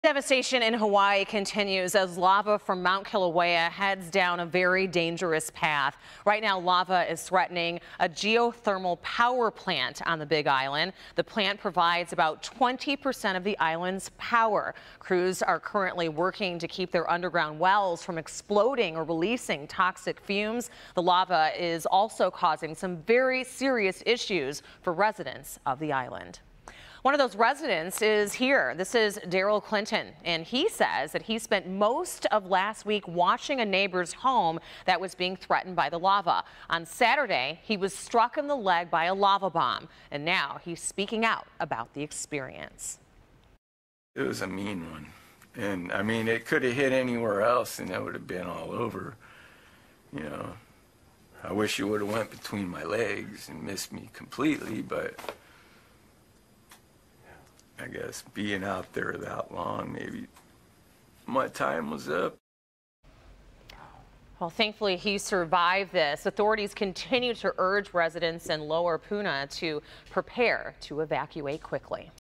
Man injured by lava bomb in Hawaii speaks out
Residents are preparing to evacuate quickly.